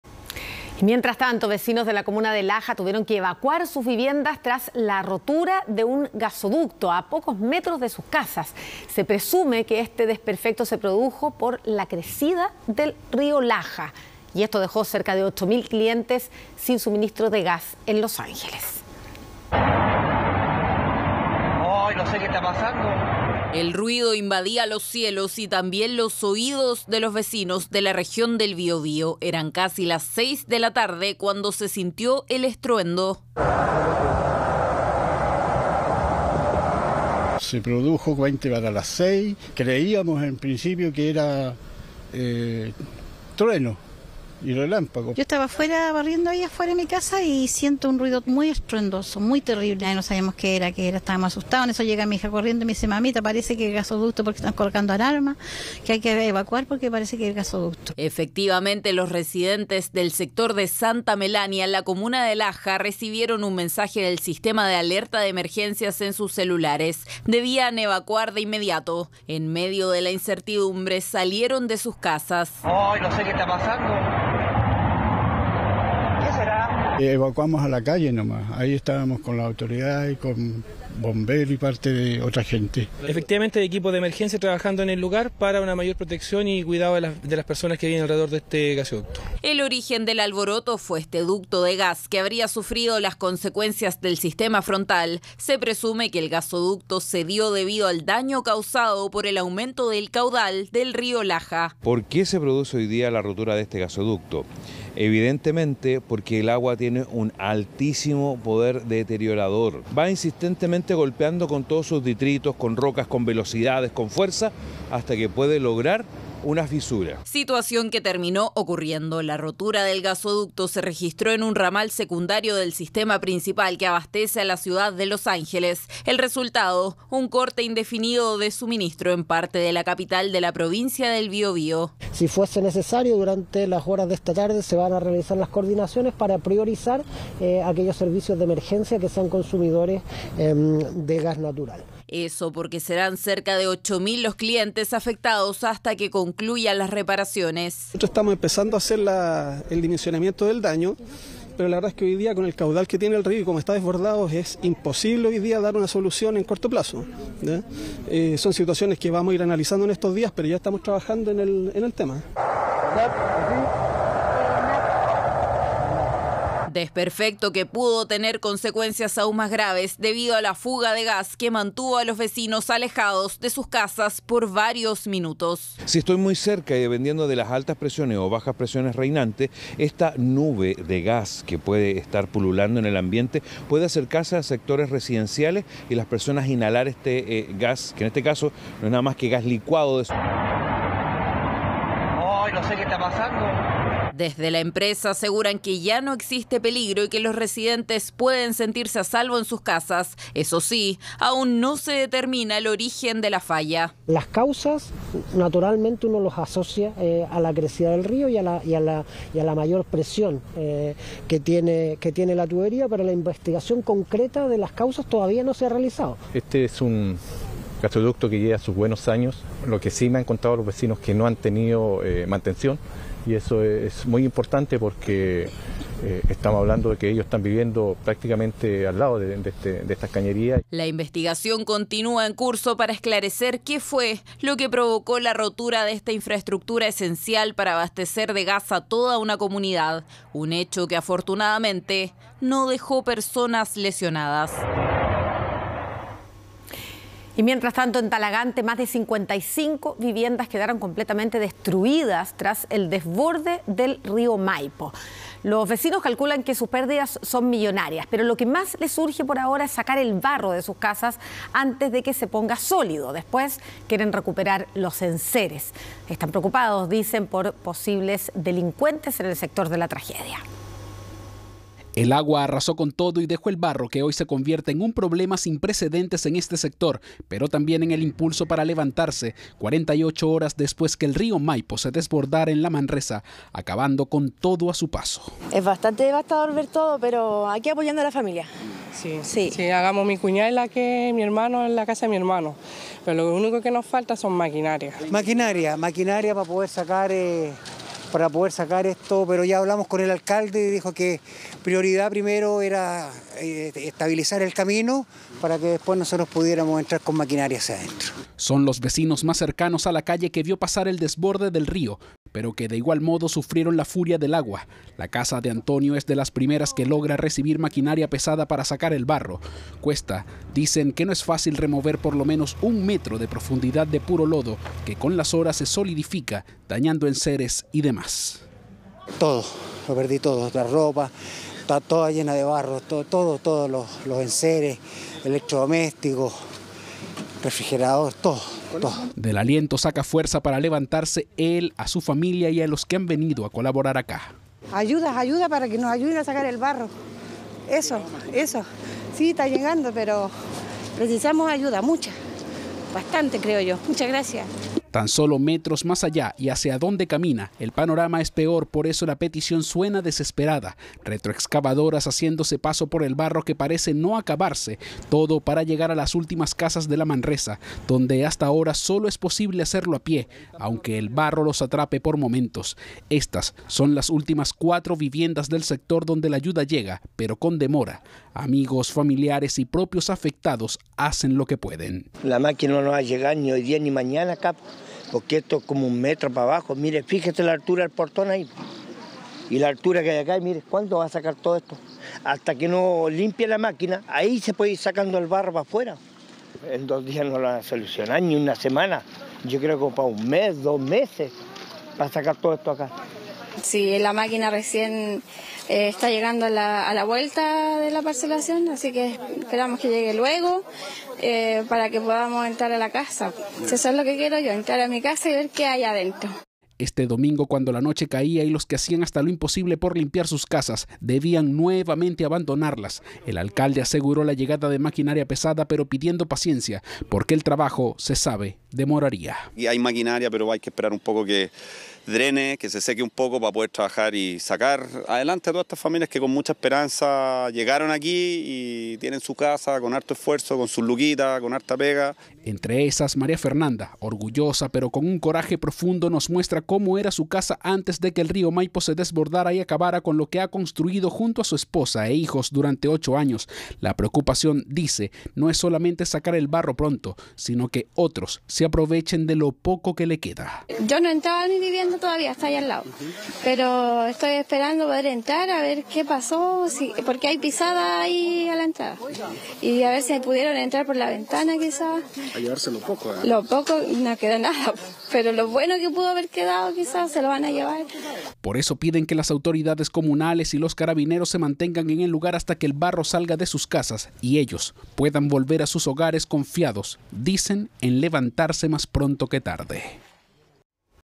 Rotura de un gasoducto obligó a vecinos de Laja a evacuar sus viviendas - CHV Noticias
Vecinos de la comuna de Laja debieron evacuar sus viviendas tras la rotura de un gasoducto a pocos metros de sus casas, Se presume que el desperfecto se produjo por la crecida del río Laja. El hecho dejó a cerca de ocho mil clientes sin suministro en Los Ángeles.

Periodista: Fernanda Jure